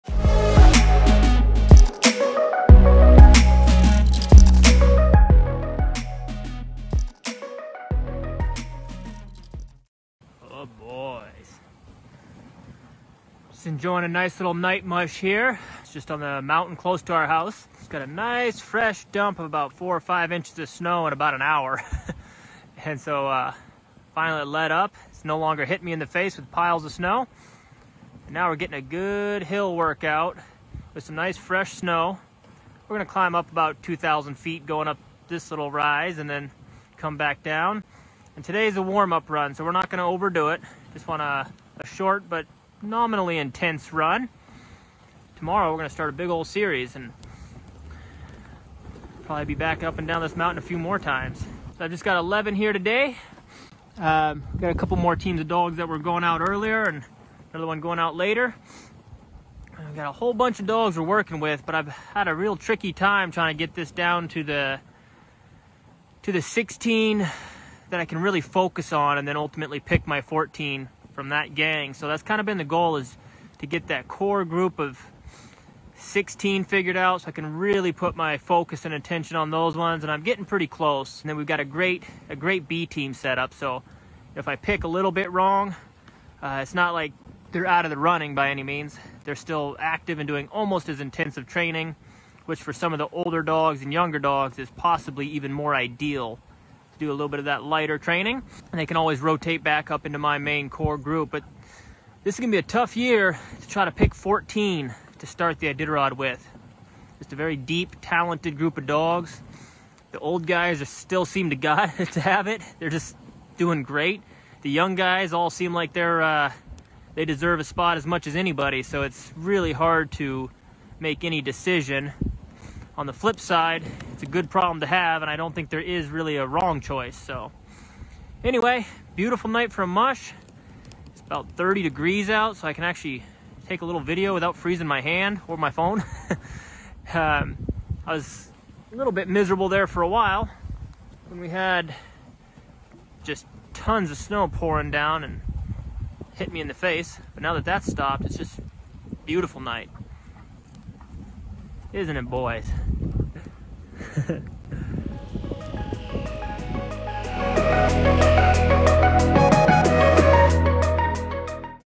How I Pick My Dogs for the Iditarod
16 Dogs. 1000 Miles. 20 check points. 1 Goal. It has been a journey to this point, but we have just over a week before the start of Iditarod 2021. Looking forward to updating you on team next week and am thankful for all of the support!